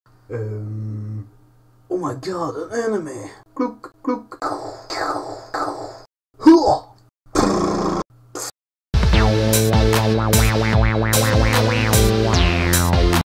paint cool this is serious
story!! chuck norris comming back from the future to kill the diablo and this is not going to be nice!